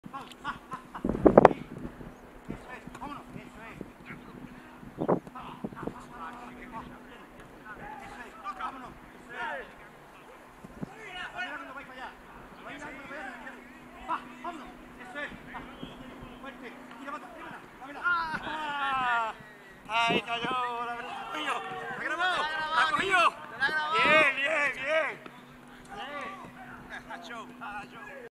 TRAINING | Jaylan Hankins takes a tumble as the goalkeepers are put through their paces
Footage from the goalkeepers' training drill...